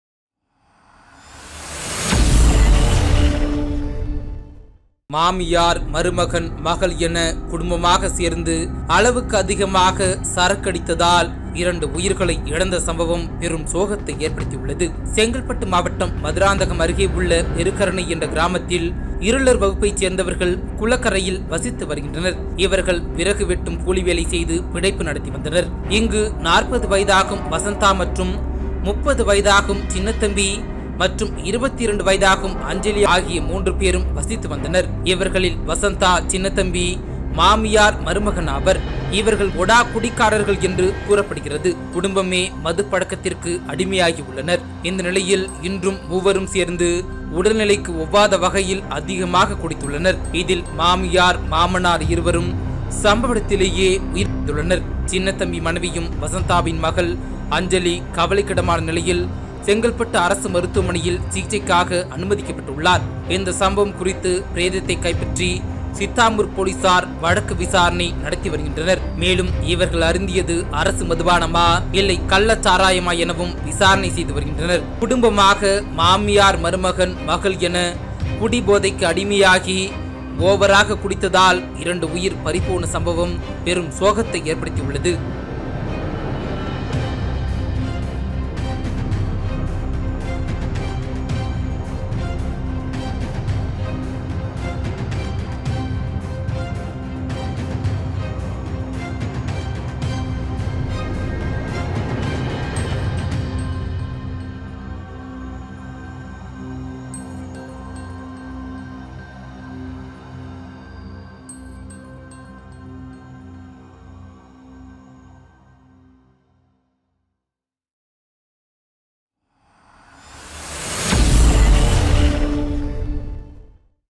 மாமியார், மருமகன், மகள் என கூட்டாக OVER குடி.. -மூவரில் இருவர் காலி.. - அதிர்ச்சி சம்பவம்!
மாமியார், மருமகன், மகள் என கூட்டாக OVER குடி.. -
மூவரில் இருவர் காலி.. - அதிர்ச்சி சம்பவம்!